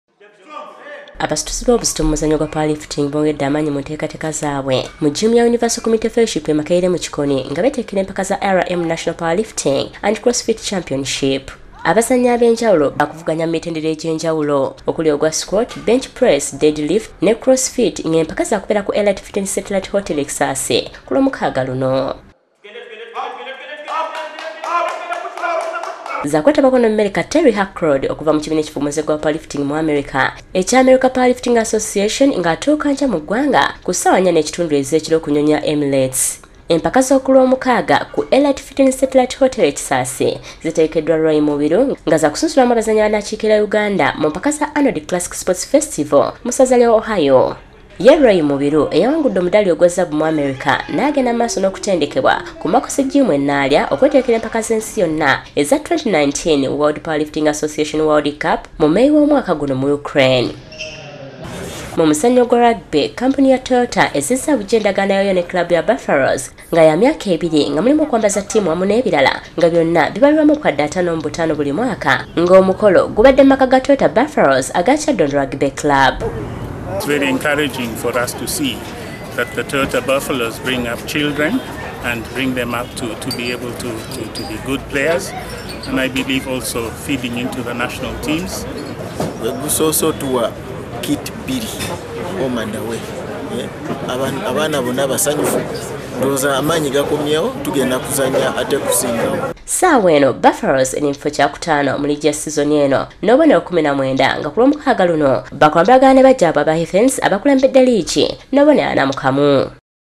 Agataliiko: Empaka za RM National Championship za Lwamukaaga.
Omumerika Terry atuuka nkya., Mubiru yeetegekera World Cup.

Lugogo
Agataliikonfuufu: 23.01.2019